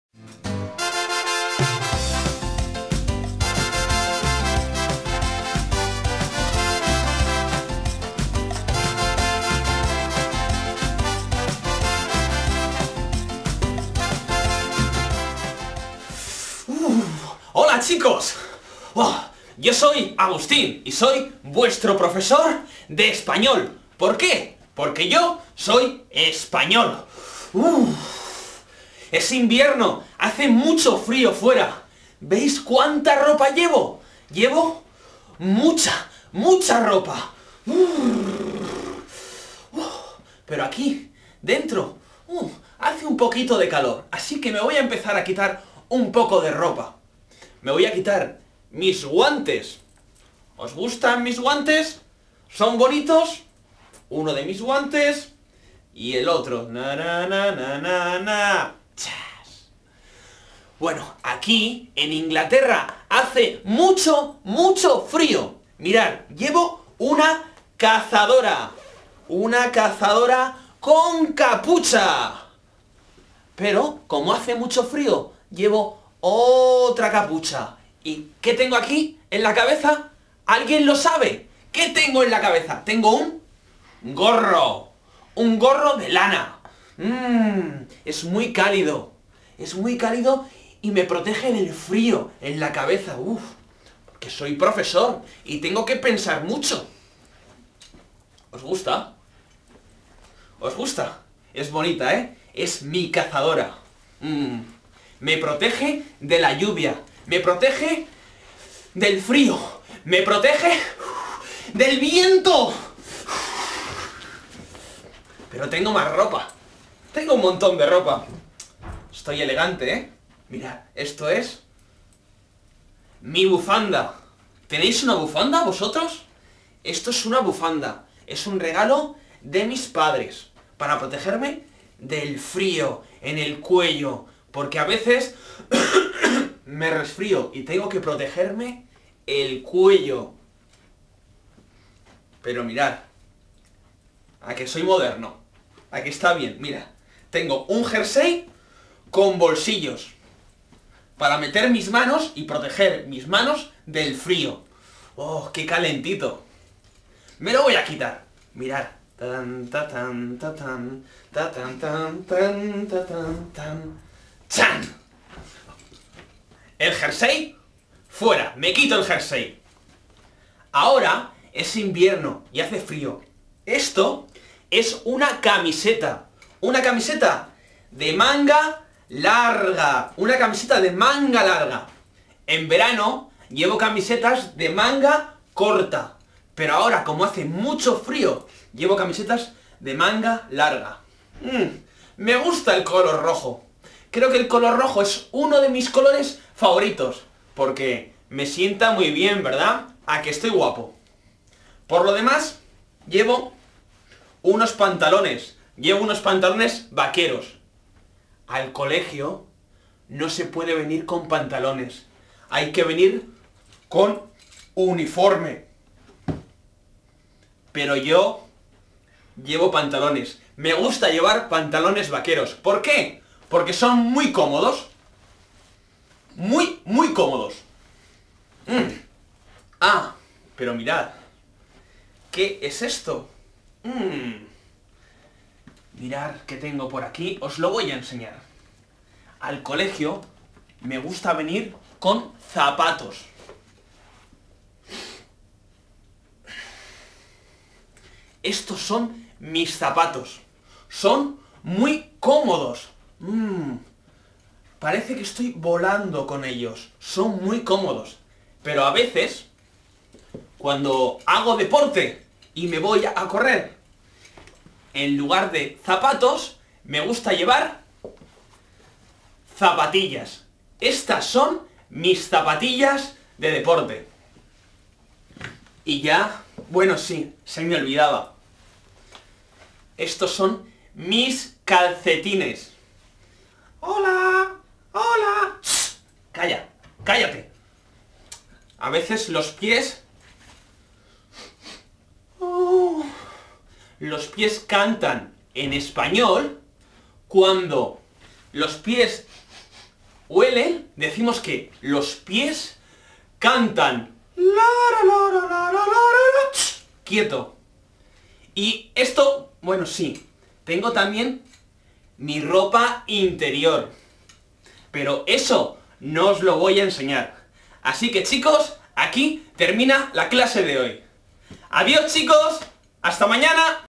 la ropa
third episode of the serie. The main topic today "mi ropa".